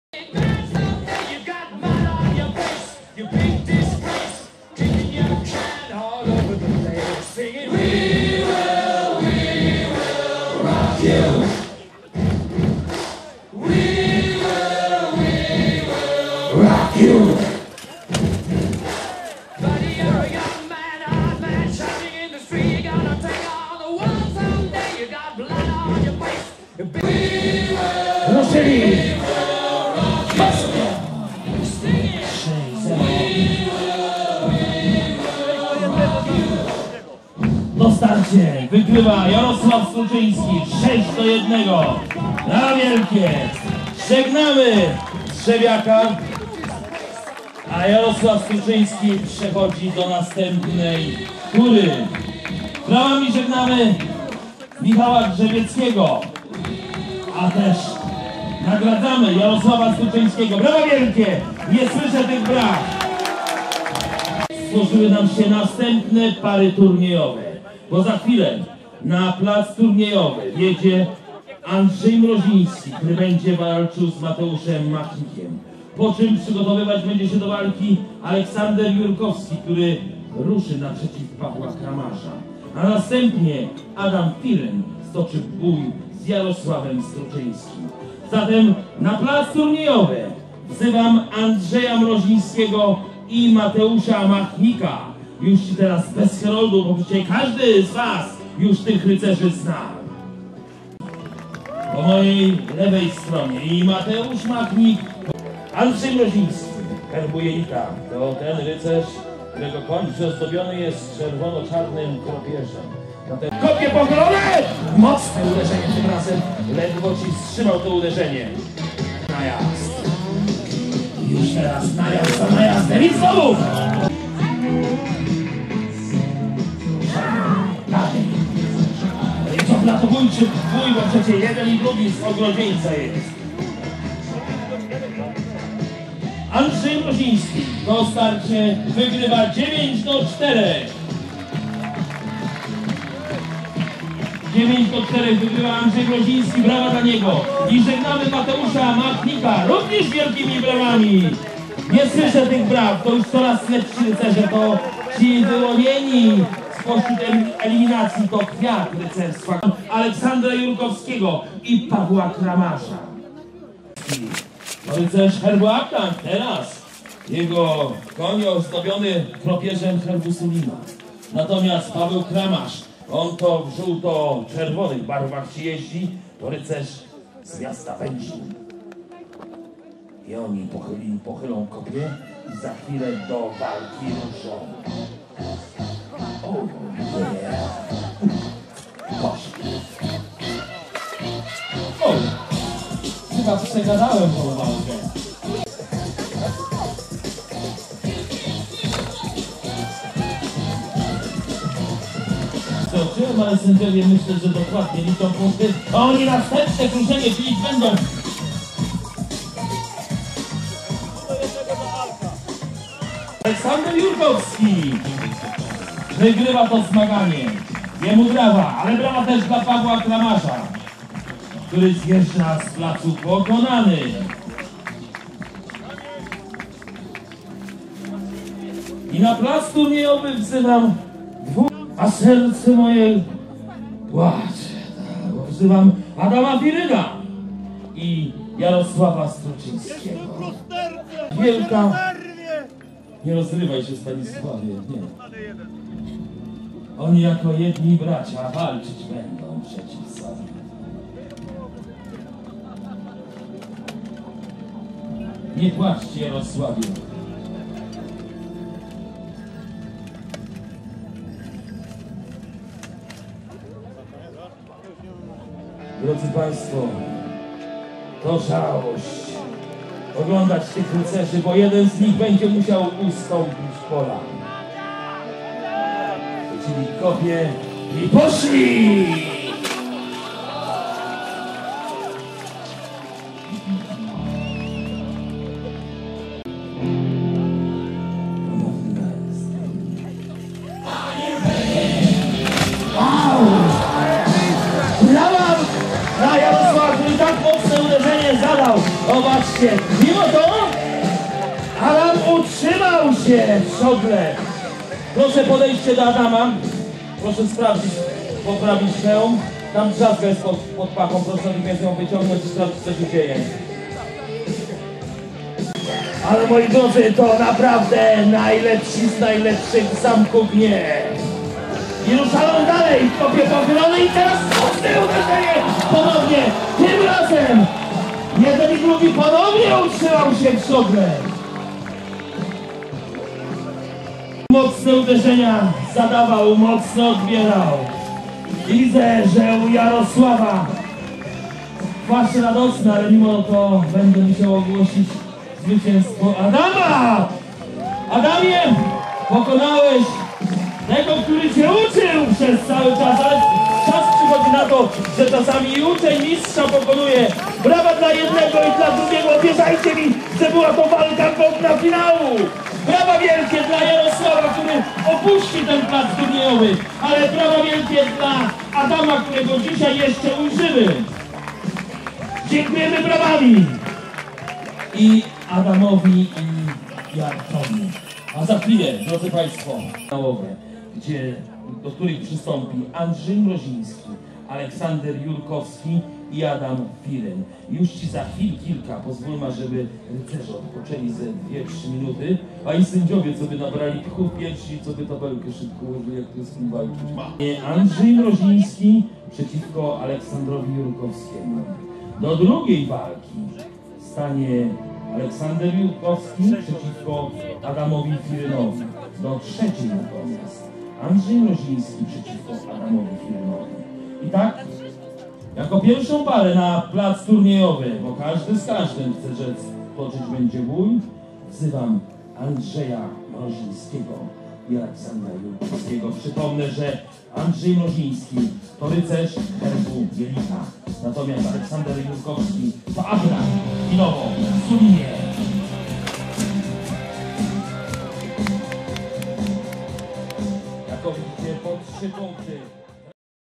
Grunwald 2011 - Turniej konny part 3
Bardzo widowiskowy, świetnie prowadzony, konny turniej na zakończenie uroczystości grunwaldzkich.
Very spectacular, well-run, equestrian tournament at the end of the ceremony Grunwald